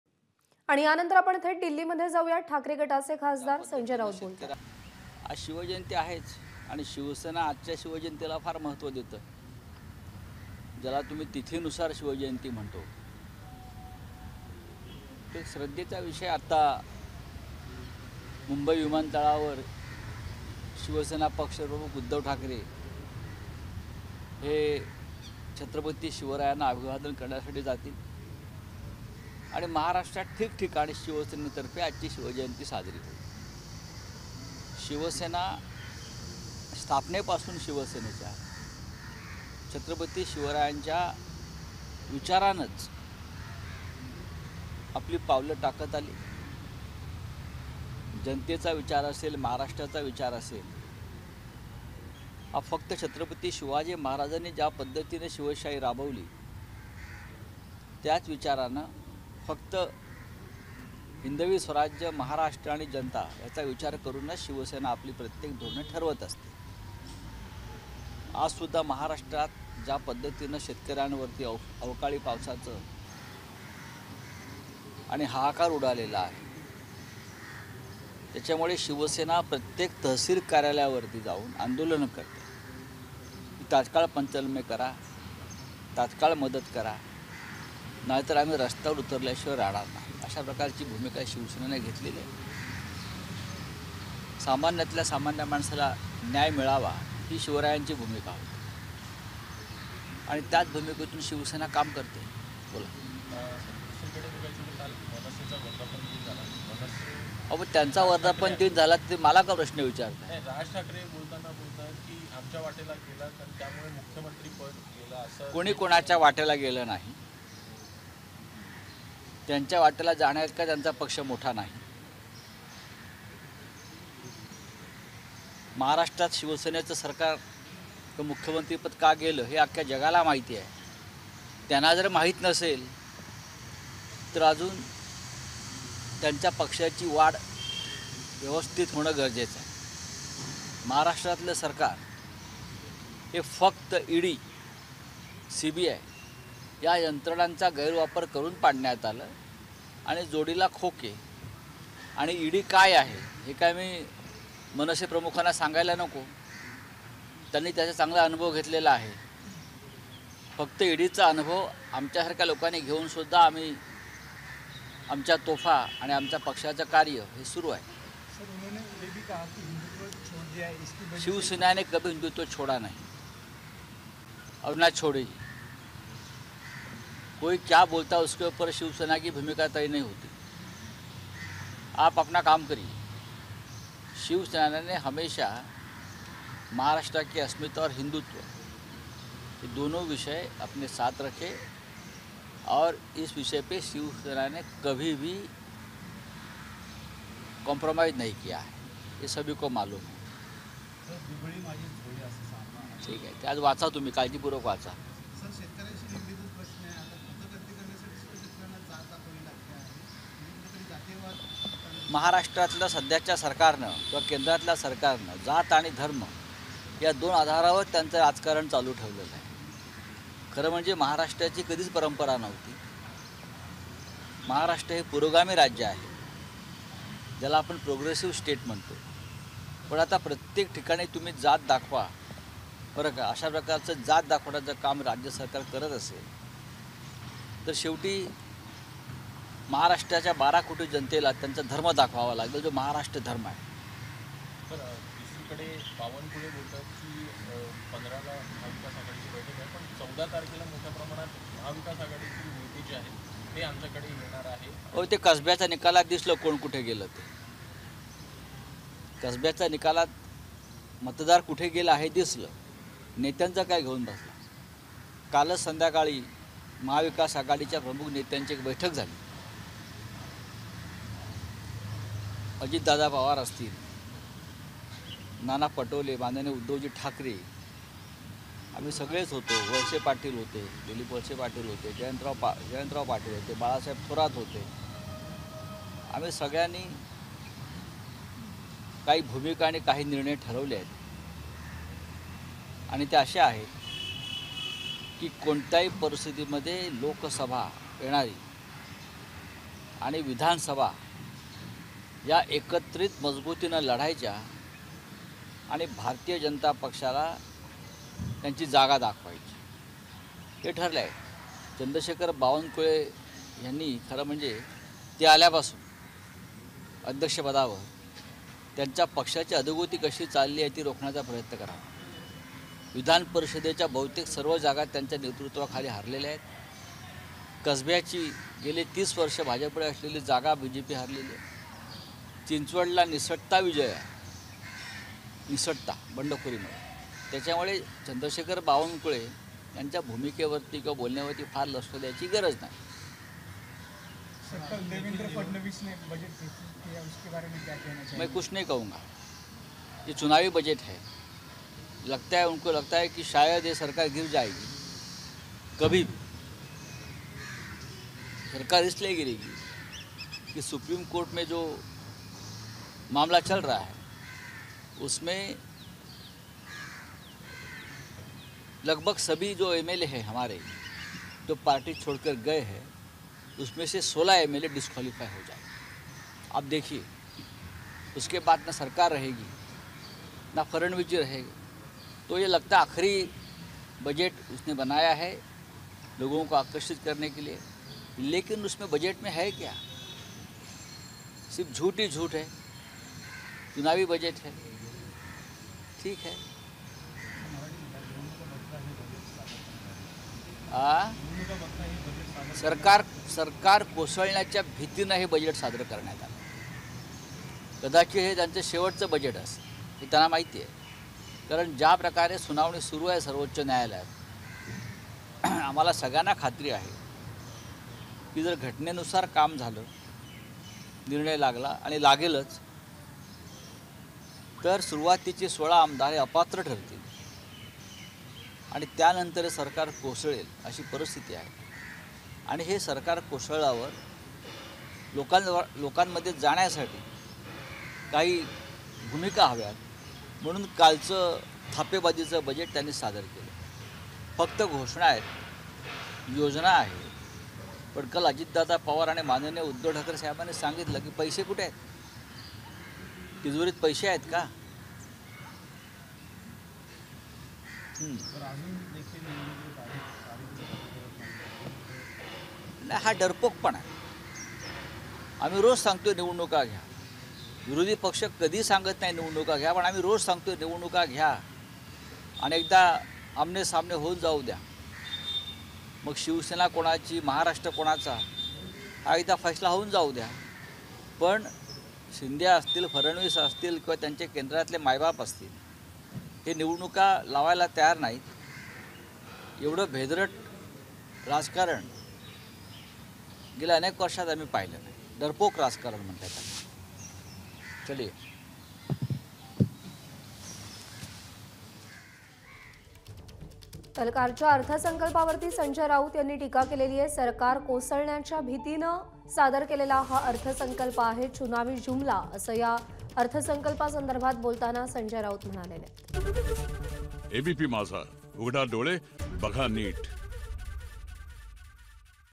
Sanjay Raut Full PC : मविआ आगामी निवडणुका एकत्रित लढणार हे ठरलंय ABP Majha
Delhi- Sanjay Raut PC FULL